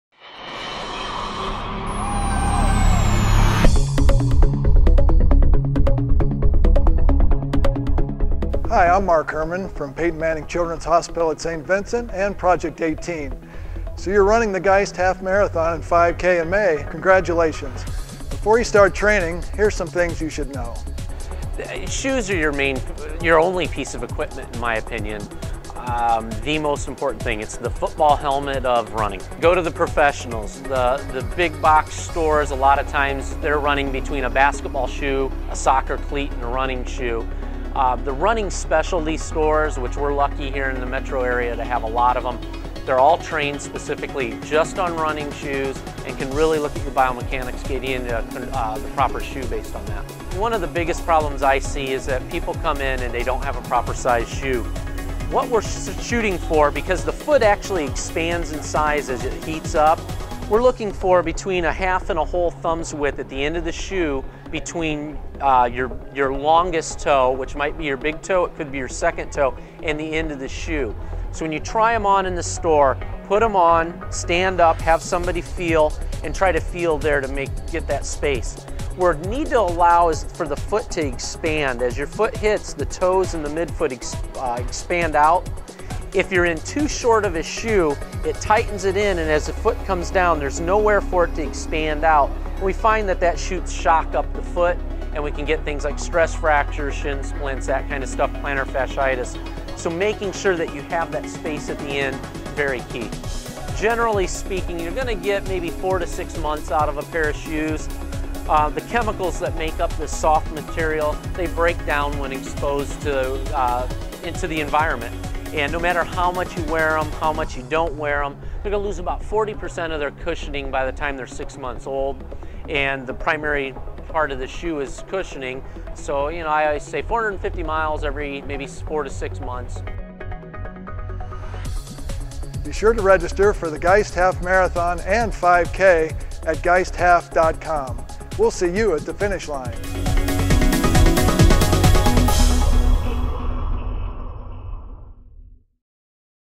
Geist Half Marathon: Episode 1
Some tips and tricks from St. Vincent's Hospital to help you find the proper shoe to train for your marathon.